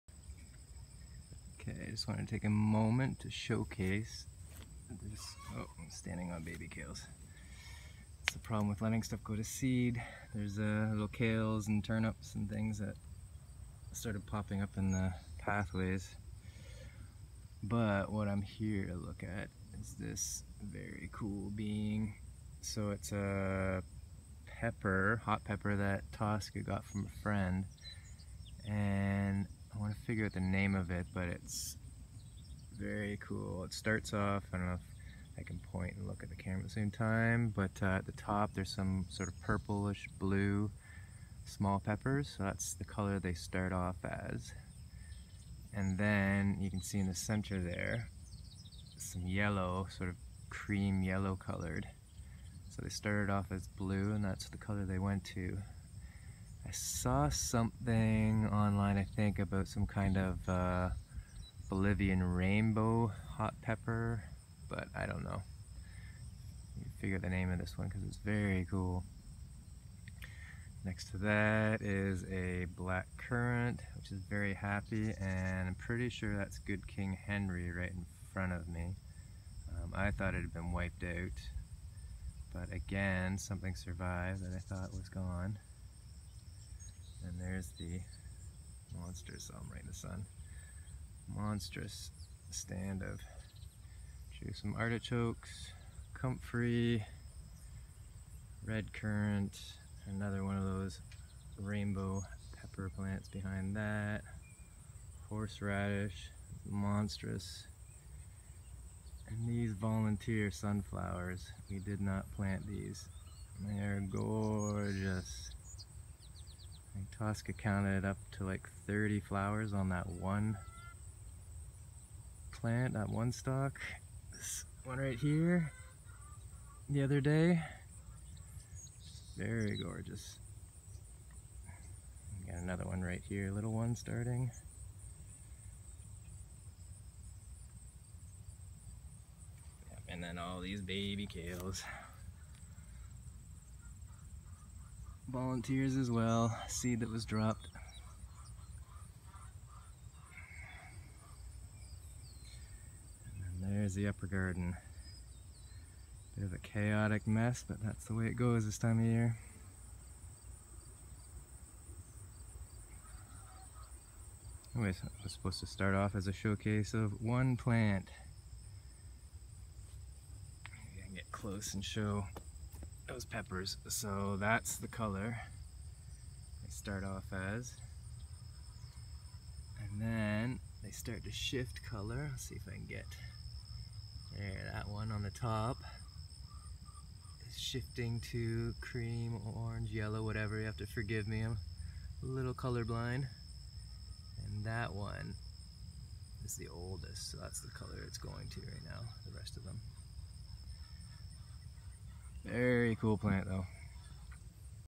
Bolivian rainbow pepper
Showcasing one of the most stunning pepper plants I've seen (or grown).